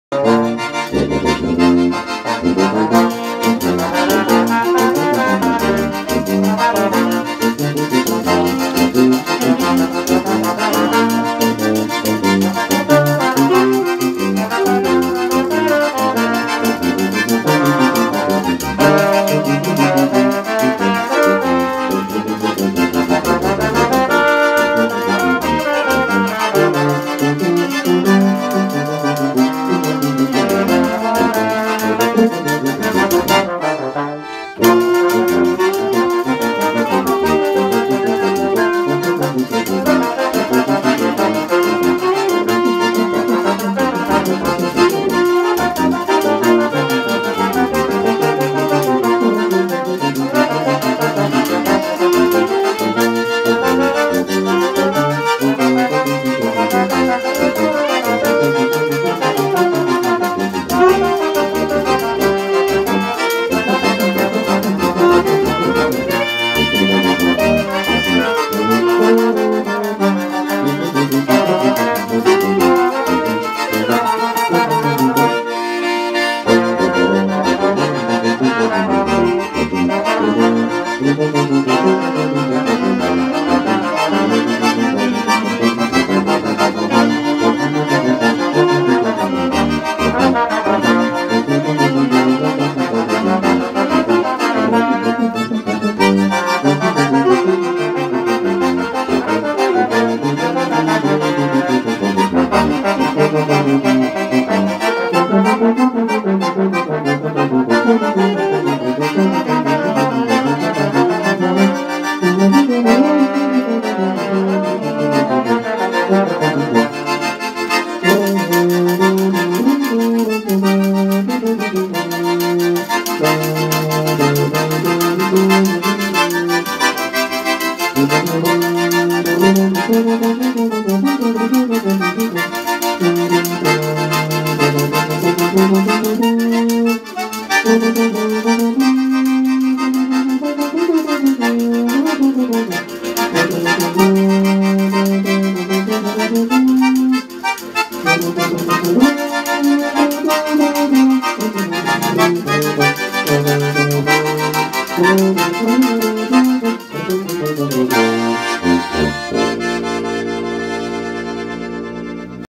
Tocata Indaiatuba / SP - 13
07/09/2017

ACORDEON - RODRIGO ( JARDIM YARA / SAO PAULO )

SAX ALTO - GUILHERME  ( JARDIM YARA / SAO PAULO )

TROMBONE - RAPHAEL  ( JARDIM YARA / SAO PAULO )

TUBA - JUNINHO ( FAZENDA PIMENTA / INDAIATUBA )

DEUS ABENCOE A TODOS E PERDOEM OS ERROS